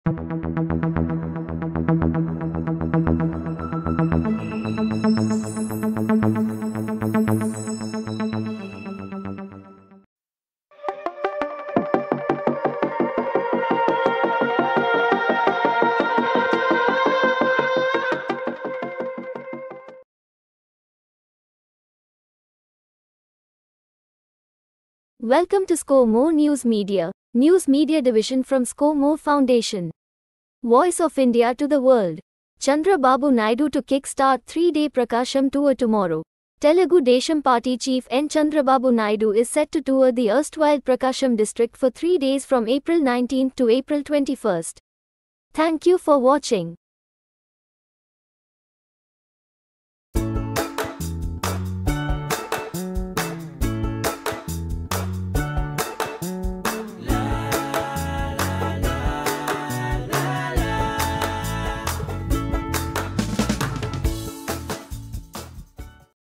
Chandra Babu Naidu to kick start 3 day Prakasam tour tomorrow
Welcome to Score More News Media, news media division from Score More Foundation!
Voice of India to the world
Telugu Desam Party Chief N Chandrababu Naidu is set to tour the erstwhile Prakasam district for three days from April 19 to April 21.